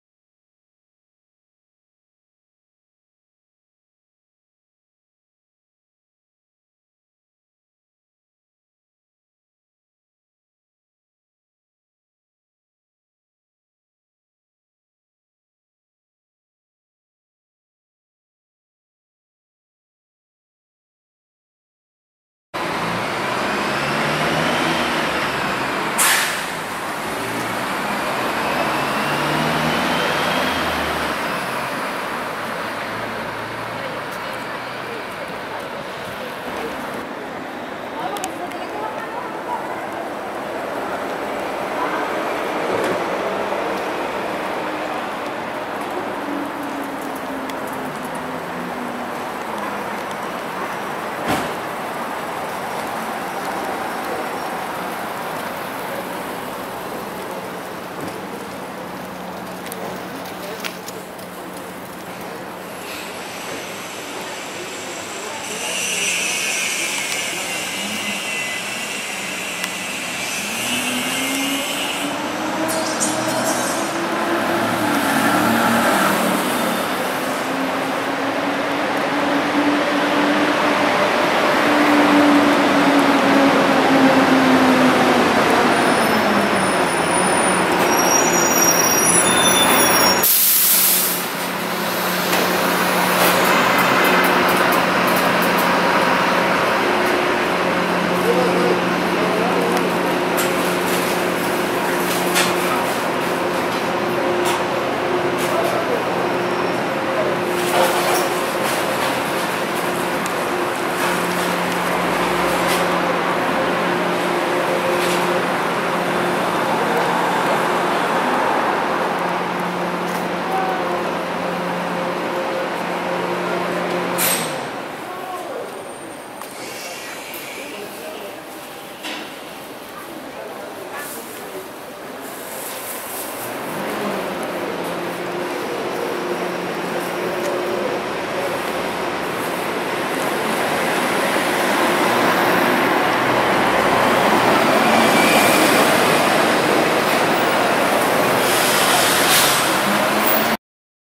Fachada teatro Municipal y calle San Antonio despues de la Romería 11 de septiembre
Santiago Centro